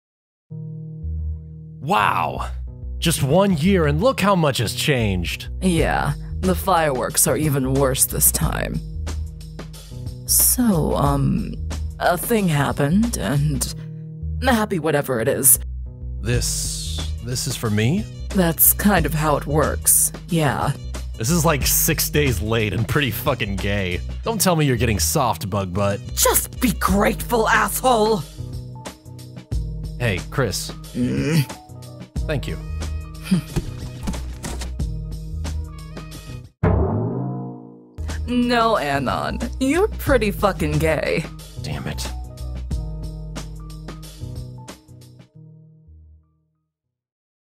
[MLP Comic Dub] Season's Givings - Comic by Shoutingisfun
A new year! And Anon and Chrysalis know just how to celebrate...

Chrysalis: Wubcake
Anon: Wuten